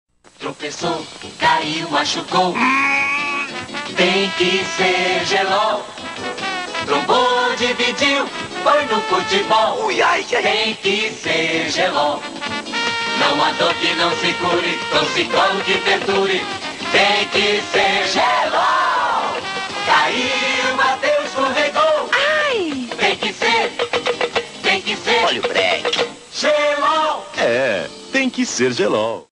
Comercial: Gelol (Anos 80)
Propaganda engraçada e inesquecível do Gelol nos anos 80. "Caiu, bateu, escorregou, tem que ser...Gelol!"